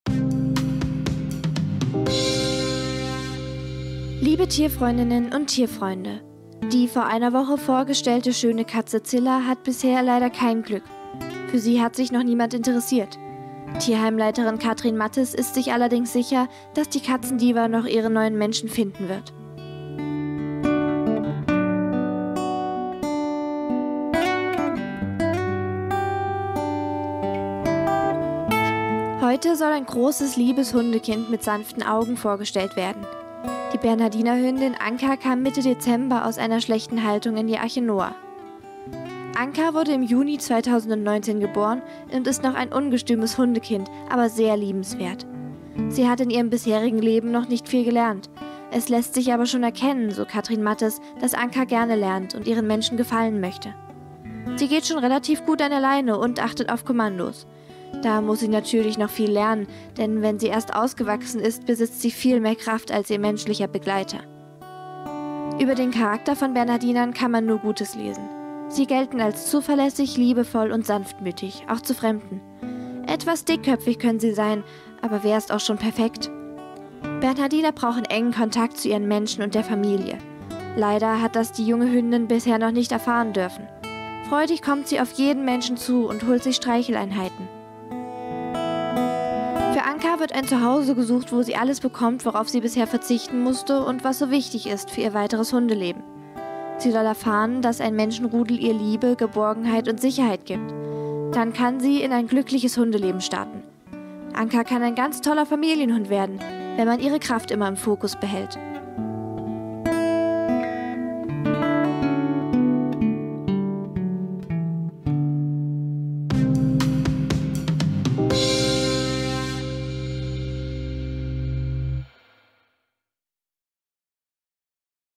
Riesenbaby Anka
Anka ist eine sehr junge Bernhardinerhündin, die aus schlechter Haltung in den Tierschutz kam. Jetzt wird für sie ein neues Zuhause gesucht, wo es ihr richtig gut geht. Mehr Informationen im Tierheim Gotha unter Tel. 03621 / 755 425.